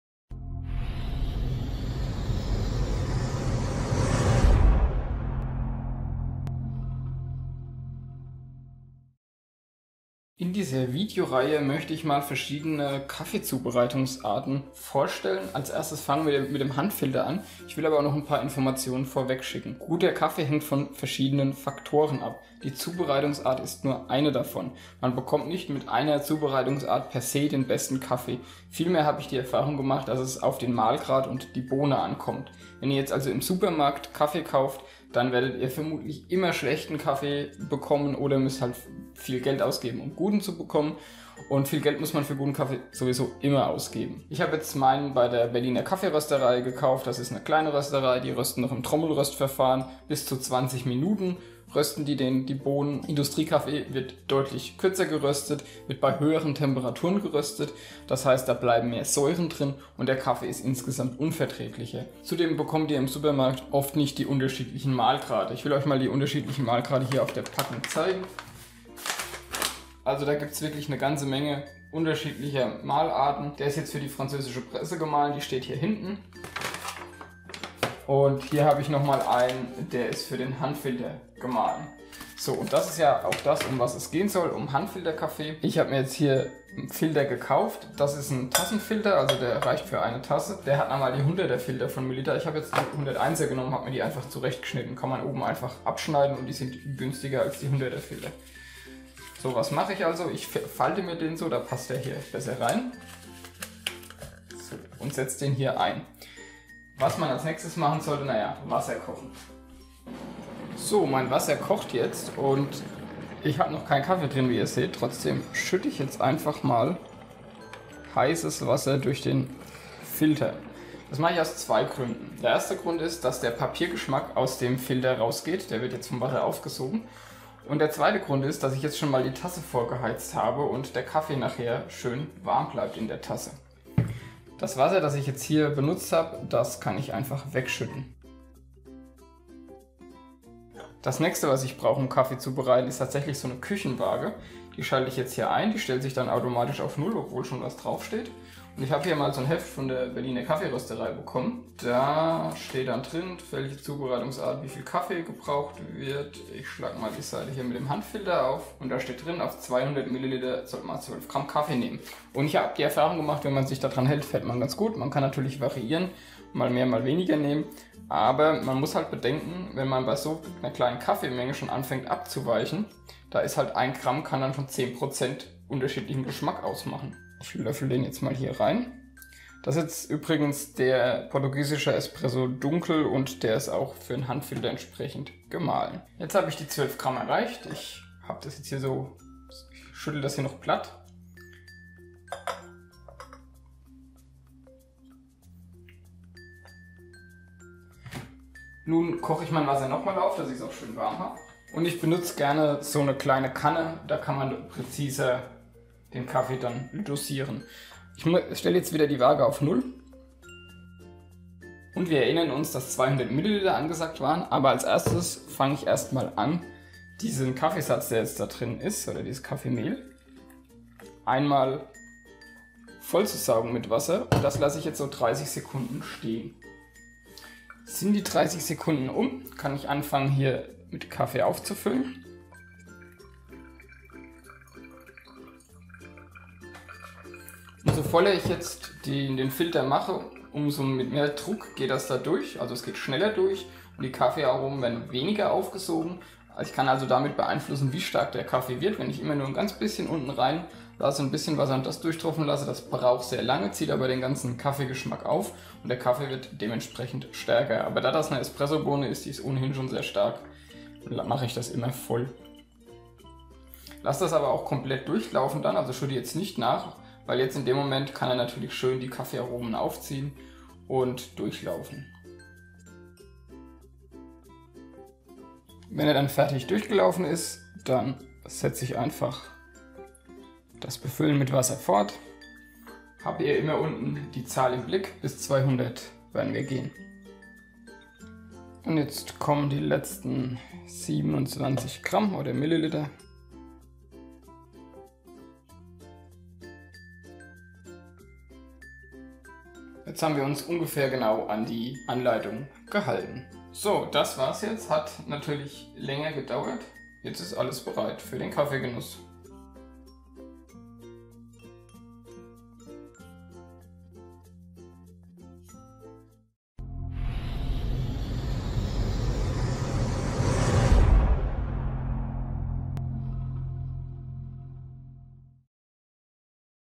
Kaffeezubereitung mit dem Handfilter
Filter (100)
Portugiesischer Espresso (ganze Bohne)

In diesem Video wird gezeigt wie man mit dem Handfilter bzw. einem Tassenfilter guten Kaffe zubereiten kann.